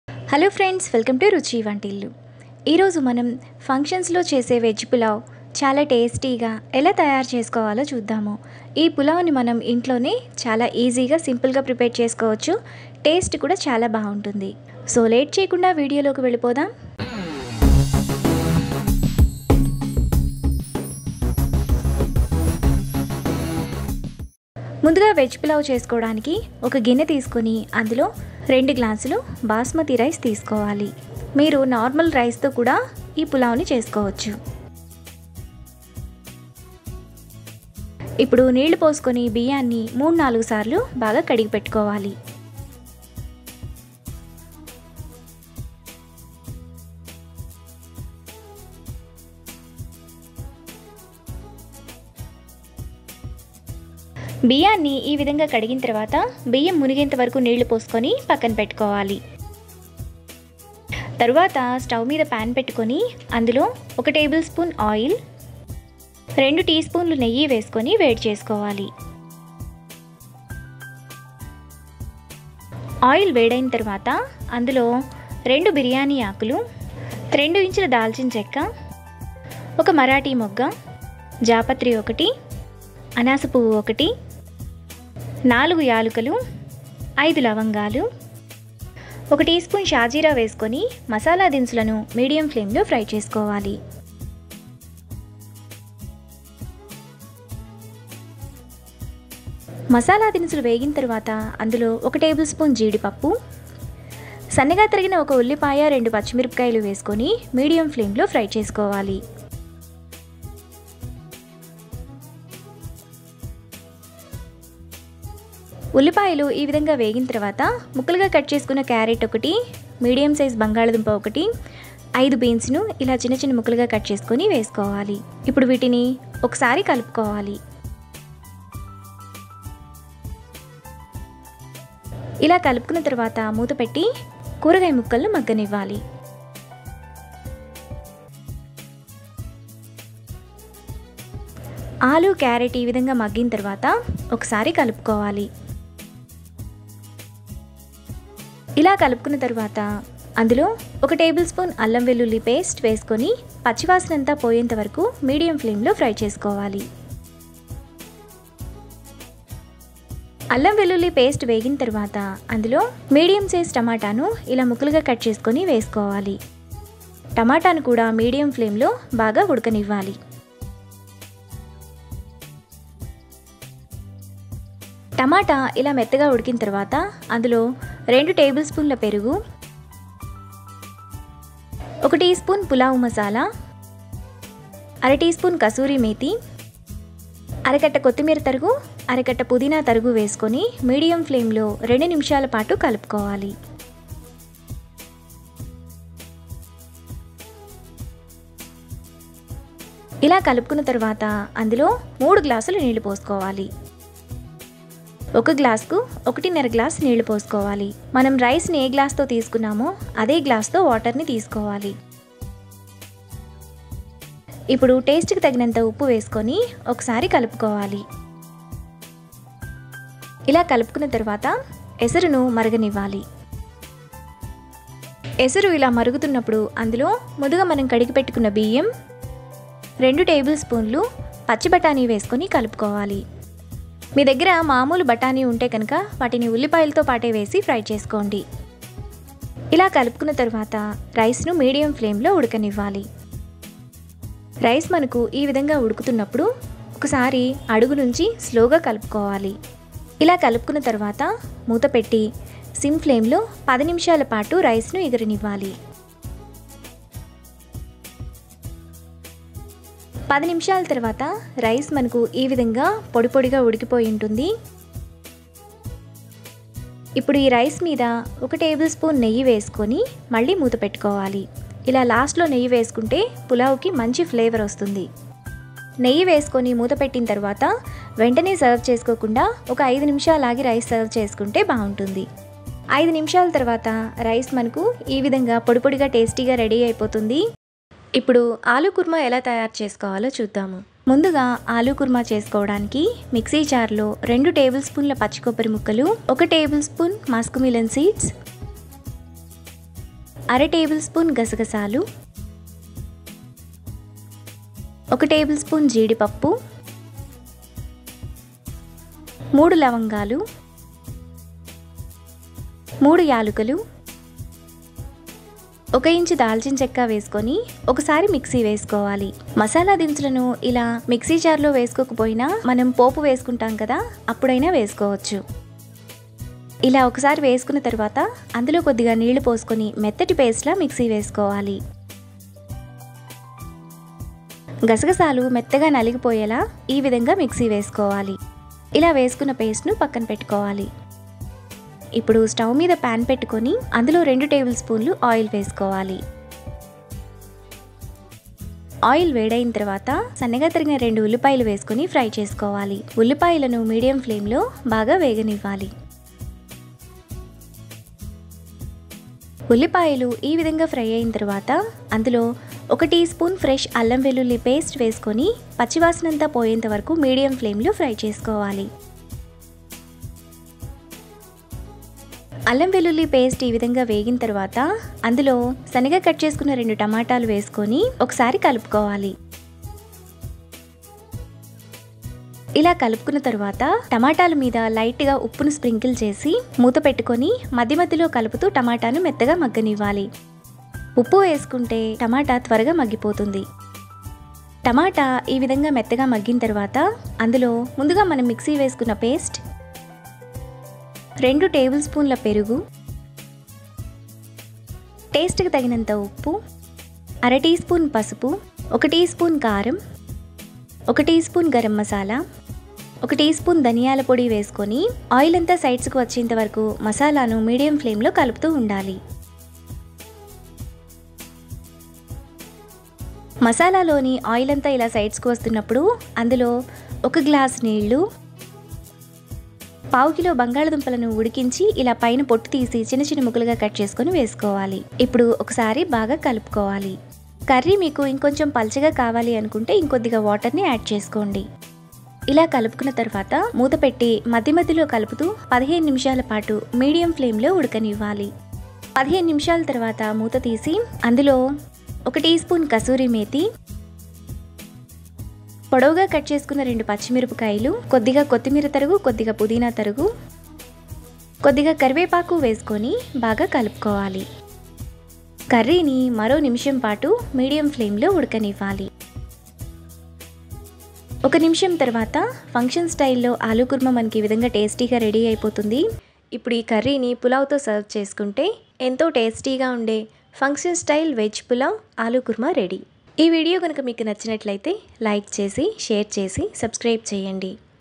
ఇంటికి గెస్టులు వస్తే ఫంక్షన్స్ లో చేసే వెజ్ పులావ్,ఆలూ కర్రీ రుచిగాచేయండి కాంబినేషన్ సూపర్ గఉంటుంది
how to make function style veg pulao and aloo kurma recipe super tasty good combination veg pulao and aloo kurma recipe making by ruchi vantillu

veg pulao
function style veg pulao
aloo kurma